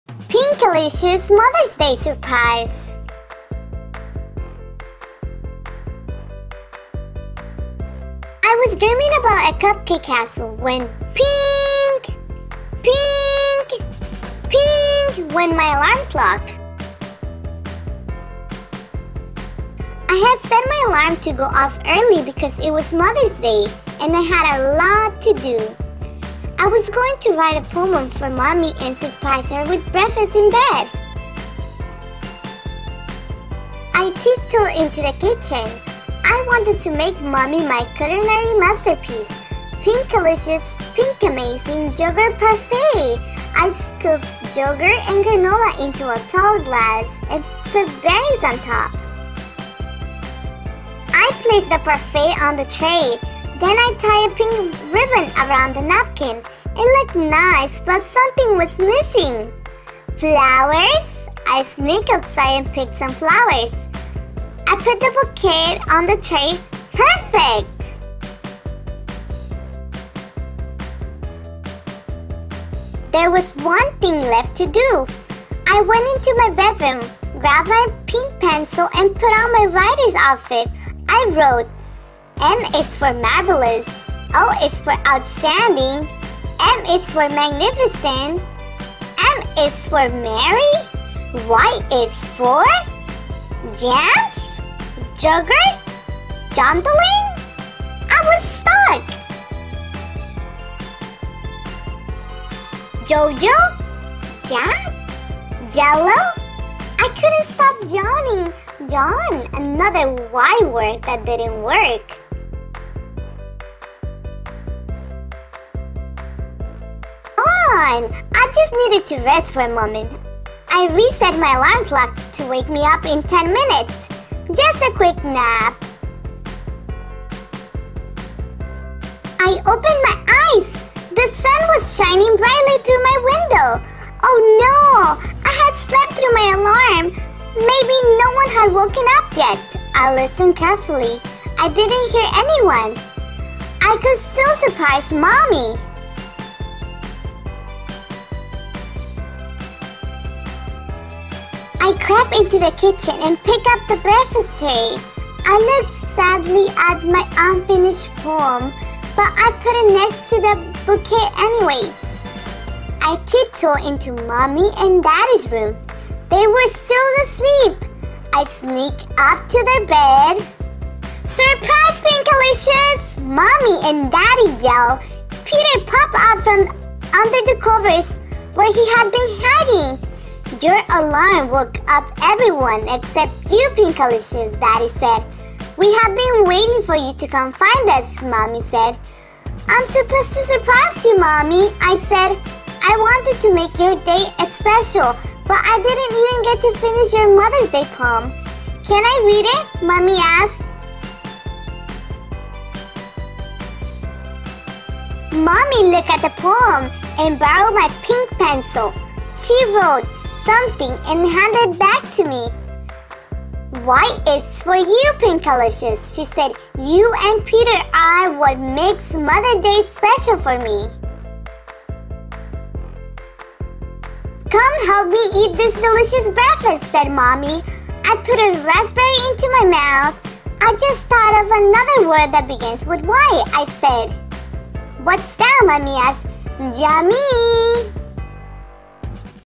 Mother's Day Read Aloud: Pinkalicious Mother's Day Surprise by Victoria Kann
Hey guys WELCOME back! to Lea's Dream Land Read Out Loud today we will be reading a Mother's Story by Victoria Kann "Pinkalicious Mother's Day Surprise".Hope you enjoy as always don't forget to Subscribe and Leave some video suggestions down below.Also Don't forget to follow us in our Facebook